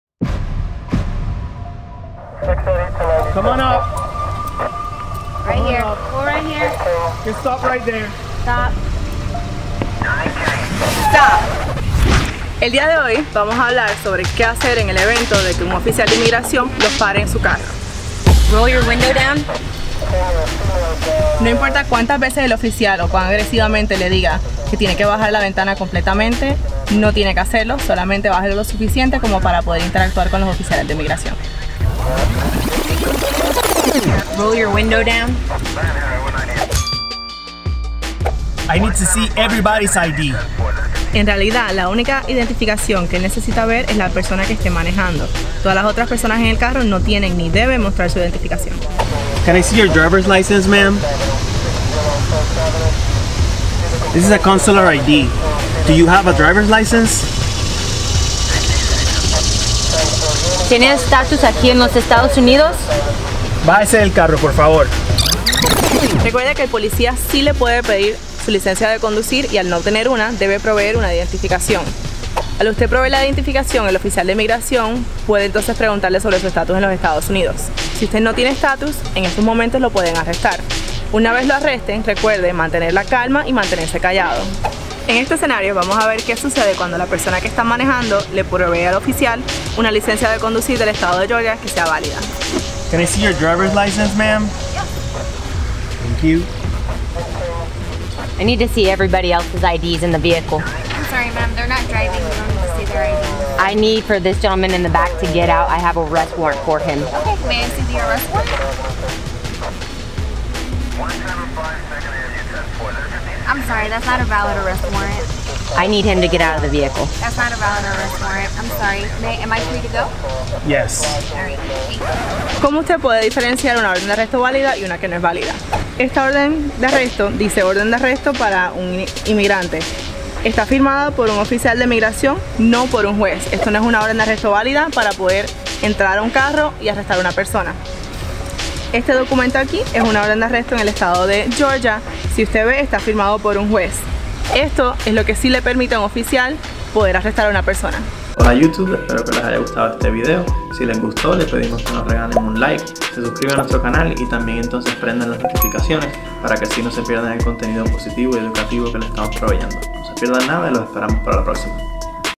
What Do I Do If I.C.E Pulls Me Over? | An Urbina Law Firm Original
We understand that driving on the road with immigration officers around can be very intimidating, but in this video, we are going to demonstrate how to properly it if you find yourself in a situation like this.

Let us know what you think of this!